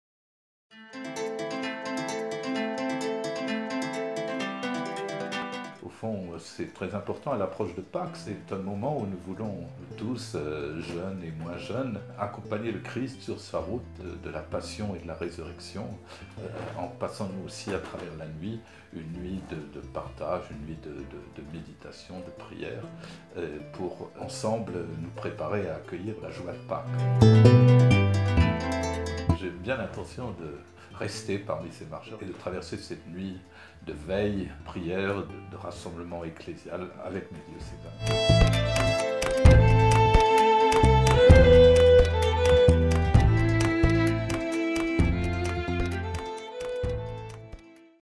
Mgr Marc Stenger lance une invitation à venir participer à la Marche vers Pâques
Le rendez-vous est lancé : prenons rendez-vous pour marcher ensemble dans la nuit sainte vers la lumière de Pâques, du 14 au 15 avril, à Troyes